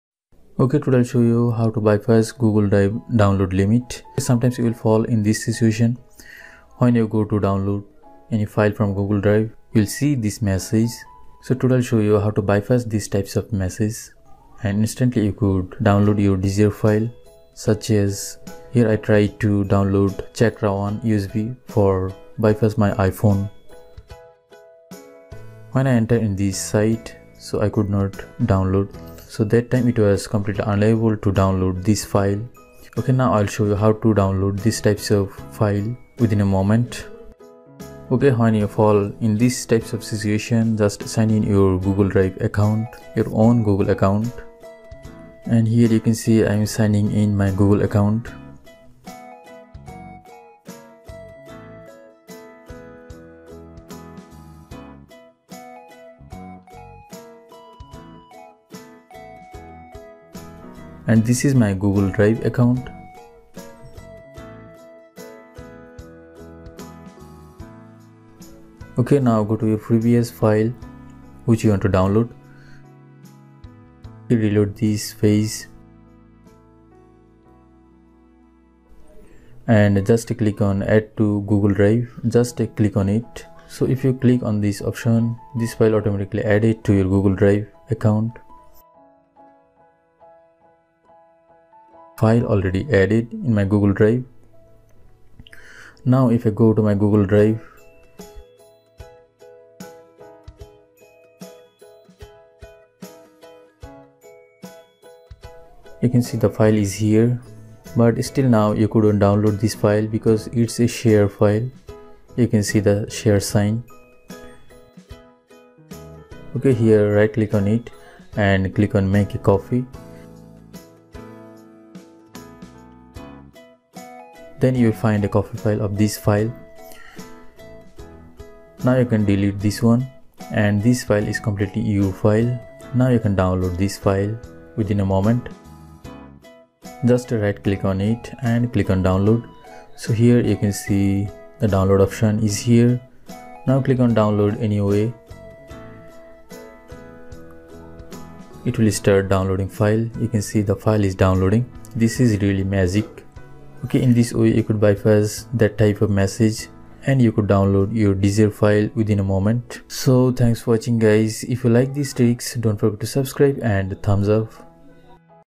google drive error fix | Can't view or download this file at this time
Can't view or download this file at this time google drive error fix just watch and bypass quota exceed google drive error.